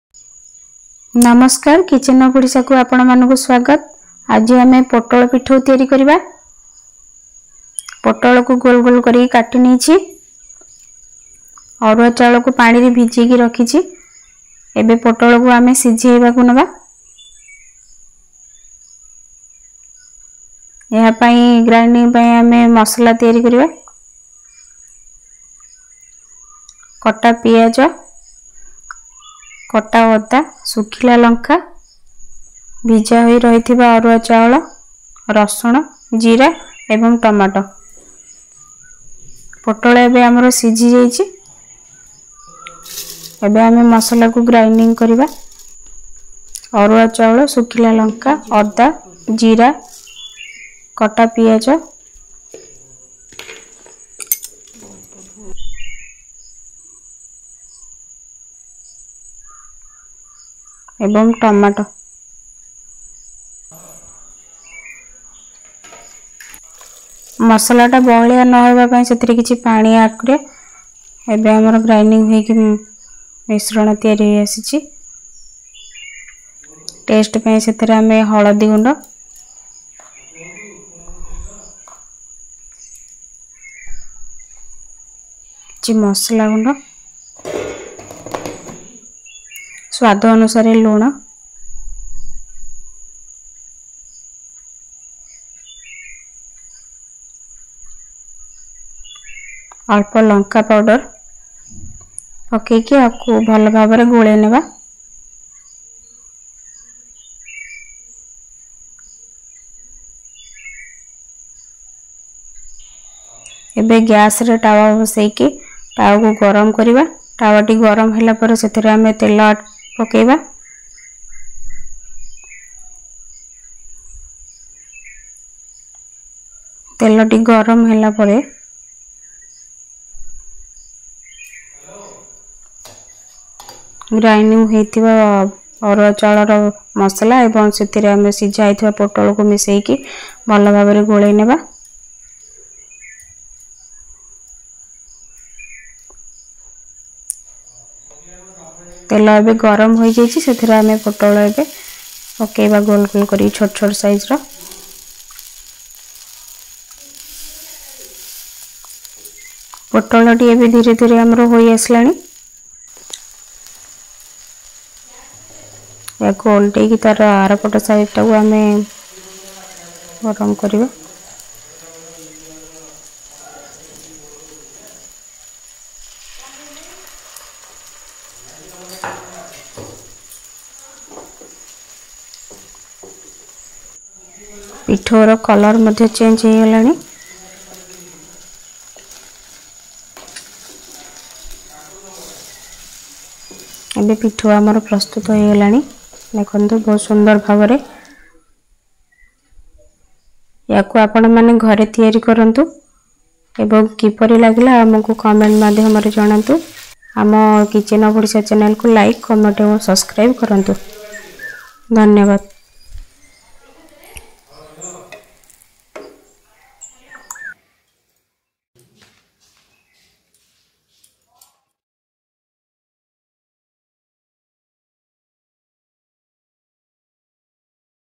ଛଚିନ୍ଦ୍ରା ପୋଟଳ ପିଠଉ | Chachindra Pithau | ସ୍ୱାଦିଷ୍ଟ ପୋଟଳ ପୀଠଉ | How to make Chachindra Pithau
ଆଜିର ରୋଷେଇରେ ମୁଁ ଛଚିନ୍ଦ୍ରା ପୋଟଳ ପୀଠଉ କରିଛି | ଏହା ବହୁତ ସ୍ୱାଦିଷ୍ଟ ଏବଂ ଆପଣ ମାନେ ଏହାର ମଜା ପଖାଳ ସହ ନେଇ ପାରିବେ| ଆପଣ ମାନେ ଘରେ ବନାନ୍ତୁ ଏବଂ କିପରି ଲାଗିଲା ଆମ୍ଭଙ୍କୁ comment ମାଧ୍ୟମରେ ଜଣାନ୍ତୁ |